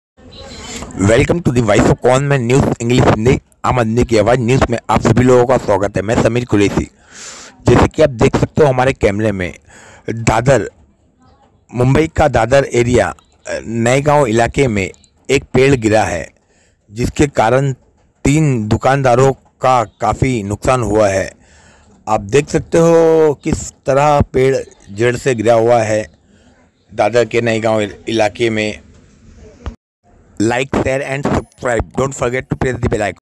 Dadar K Naigaon Mei Pedh Gira 3 Shops Damage Due To Heavy Rainfall in Mumbai, Orange Alert Till 14th
Dadar K Naigaon Area Mei Pedh Gira Is Hadse Mei 3 Shops Damage Hua Hai Due To Heavy Rainfall in Mumbai, Orange Alert Till 14th of July 2022

Reporting By: Sameer Qureshi, The Voice of Common Man English/Hindi Newspaper/Channel